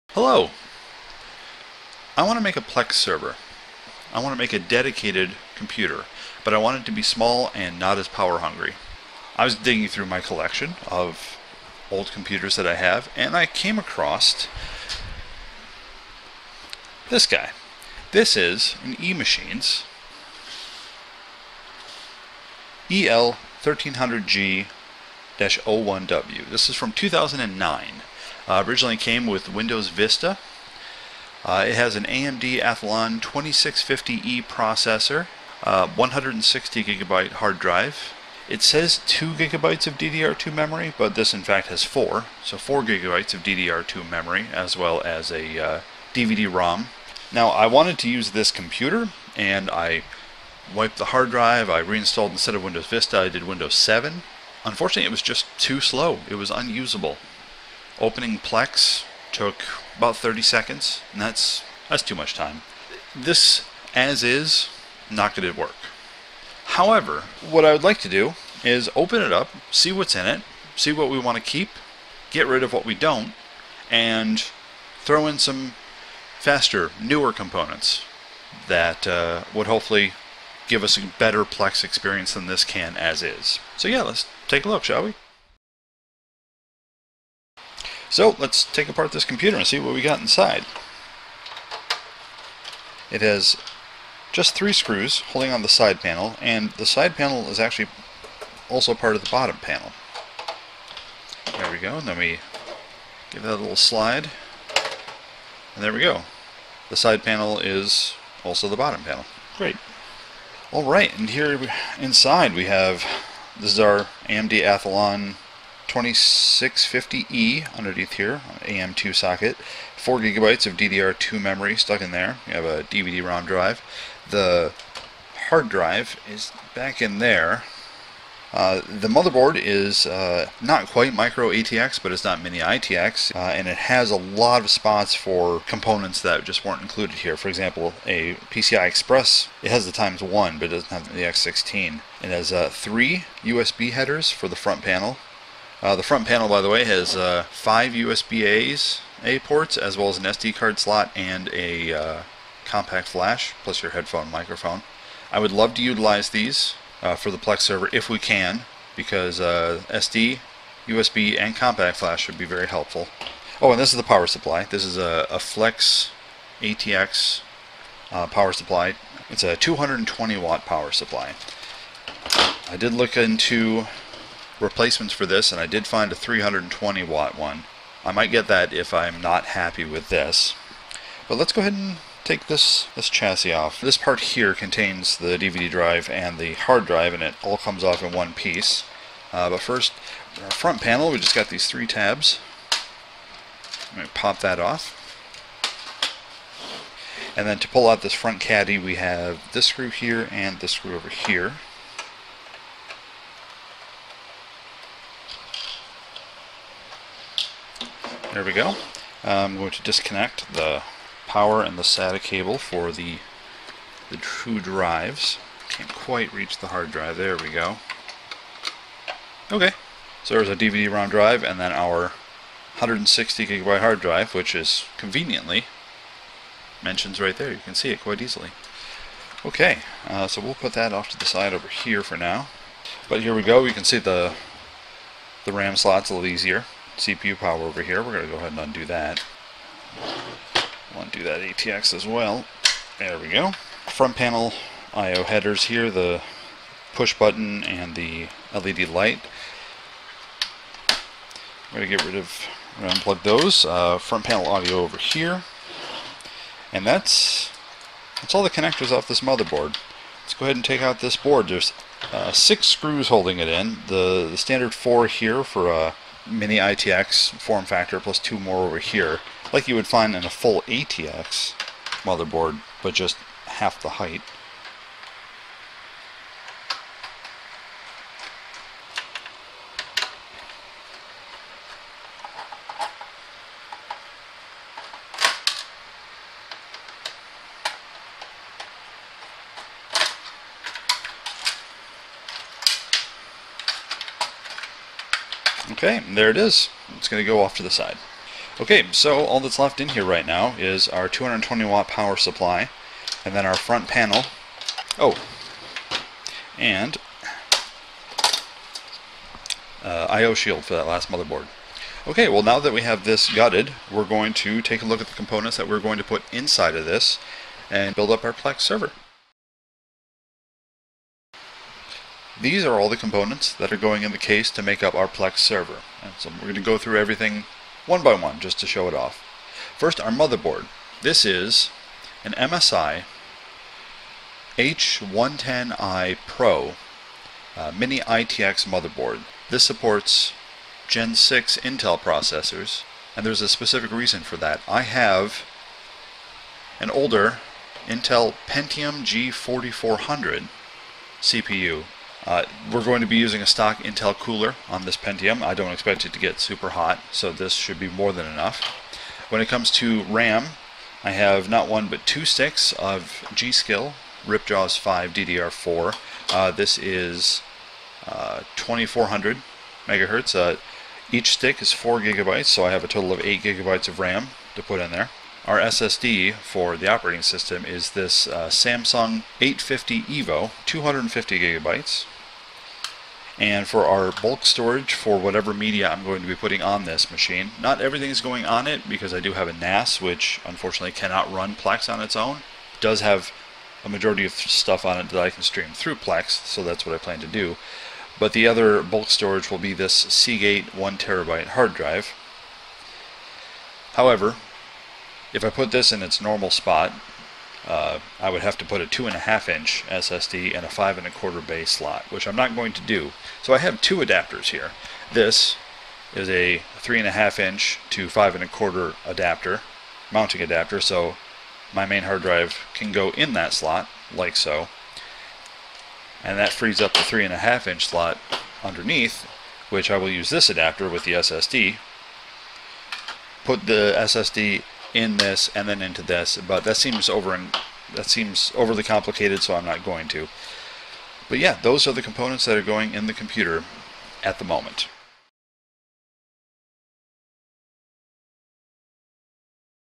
[PC BUILD] Building my first dedicated Plex server PC (PART 1)
In Part 1 of this two-part series, I will be building a PC dedicated to running my personal Plex server, using older components I had laying around.